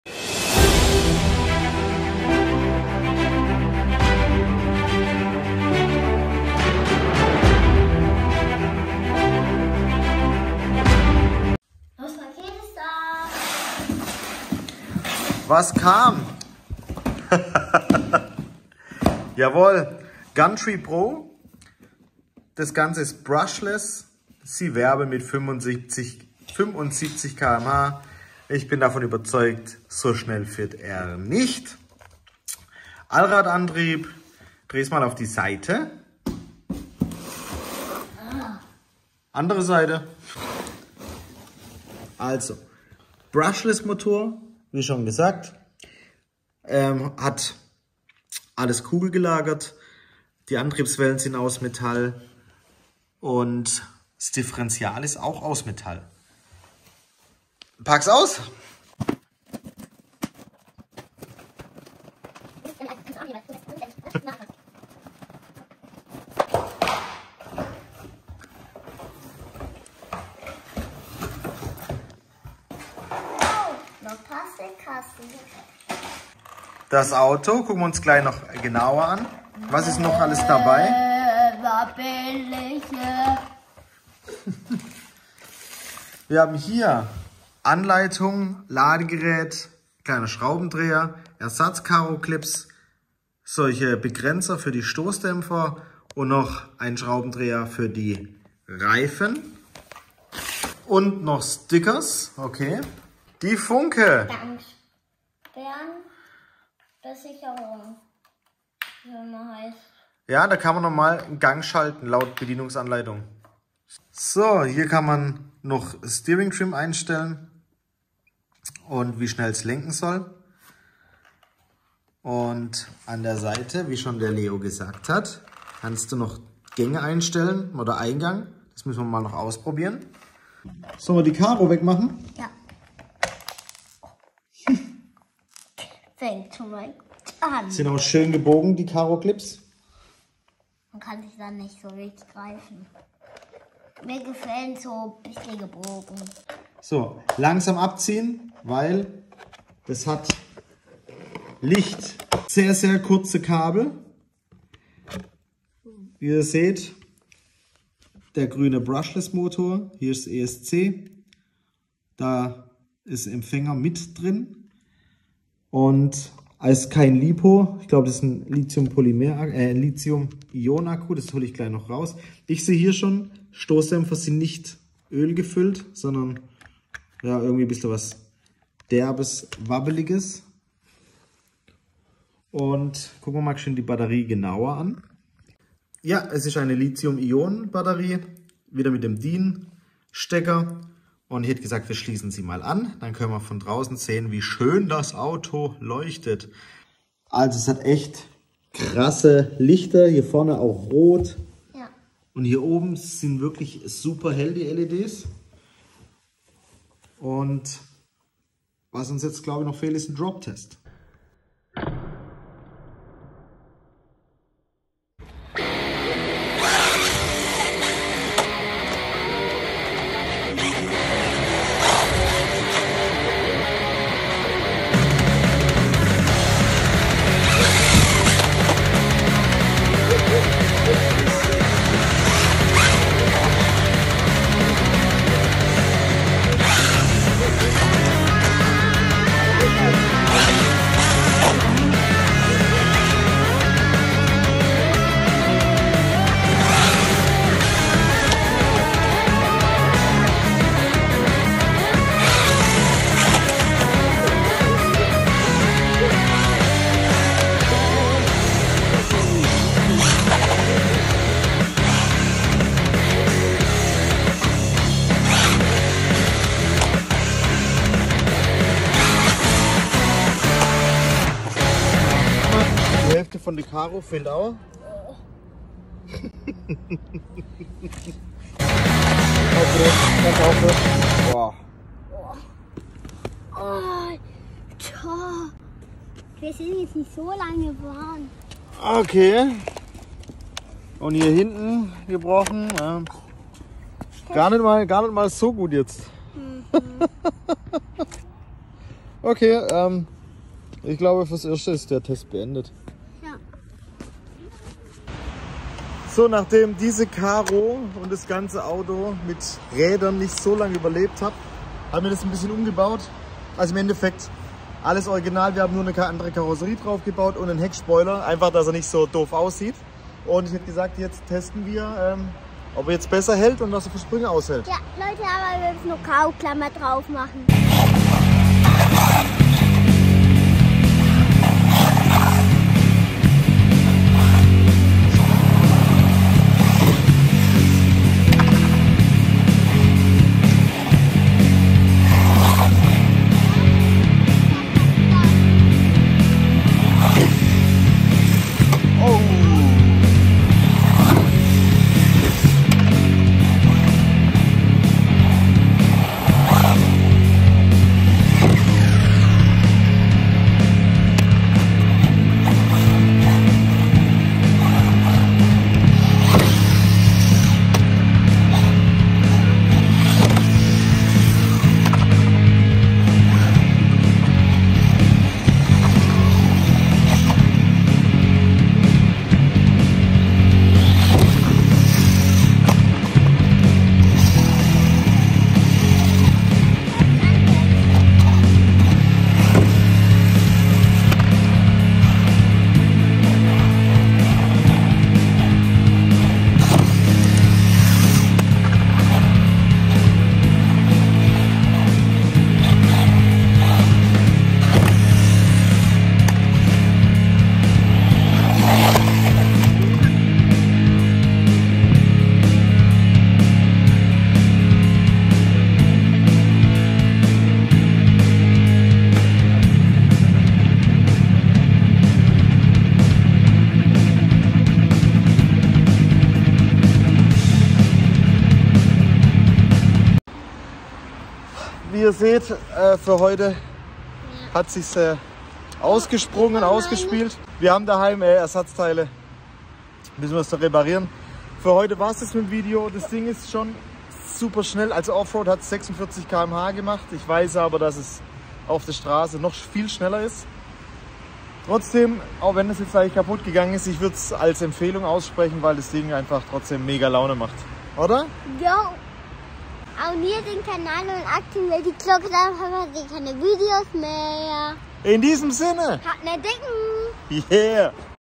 Gantry Pro Rc brushless 1/16! Überlebt aber nur 3 Minuten! Was ist die Lösung? Rc Auto/rc car
Hi freunde,

Habe den gantry pro hier zum Testen und ich muss sagen, er ist schnell aber brutal instabil 🙈
Seht selbst!

Auto gibts hier

Hab nen dikken…

Dein Leo